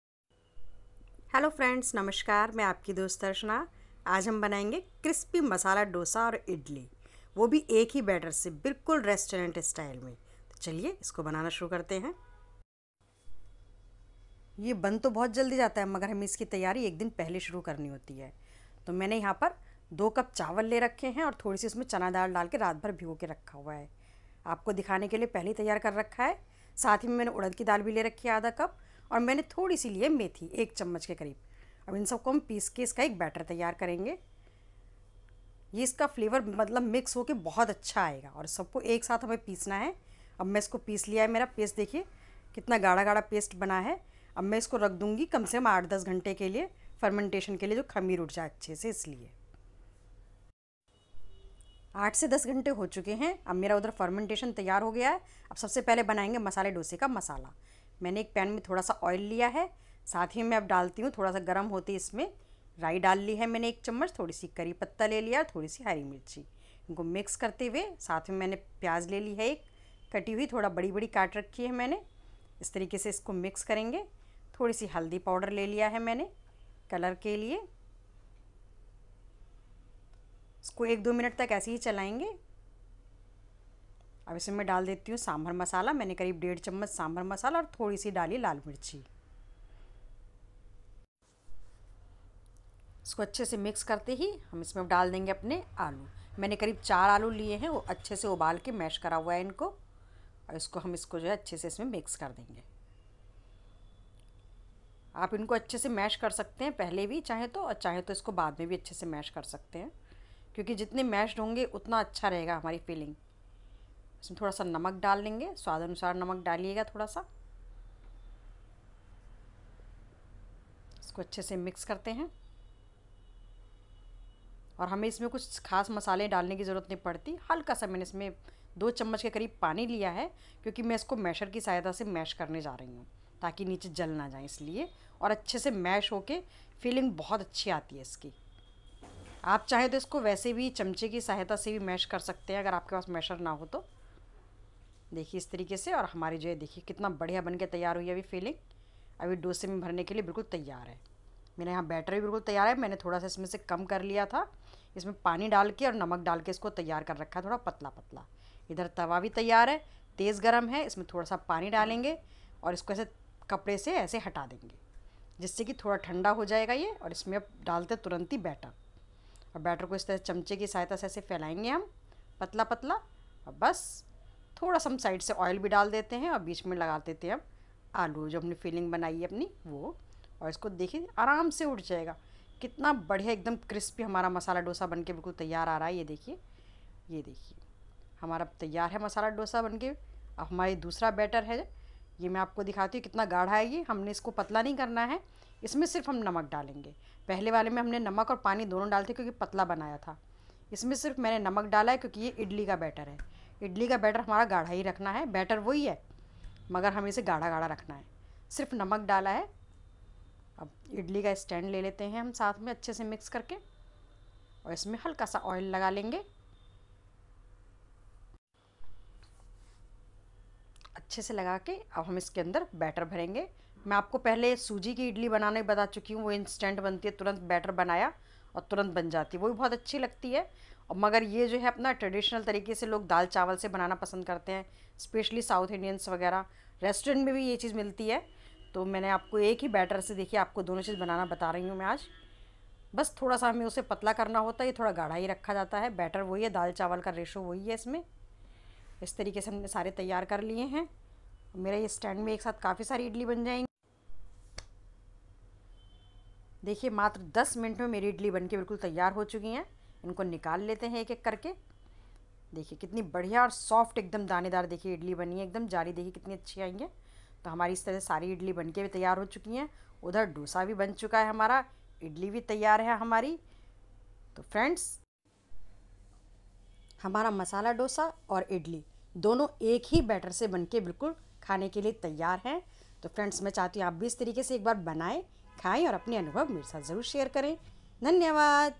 Masala Dosa & Idli || Dosa & Idli With One Batter || Easy Masala Dosa & Idli
INGREDIENTS:-

Batter:
1. Rice - 2 Cup
2. Chana Dal - 1/3 Cup
3. Urad Dal - 1/2 Cup
4. Methi Seeds - 1 Tsp
5. Salt

Aloo Masala:
1. Oil - 1 Tbsp
2. Mustard Seeds - 1 Tsp
3. Curry Leaves
4. Turmeric Powder - 1/2 Tsp
5. Green Chillis - 3-4
6. Sambhar Masala - 1 1/2 Tsp
7. Onion Chopped
8. Potatoes (Boiled n Mashed)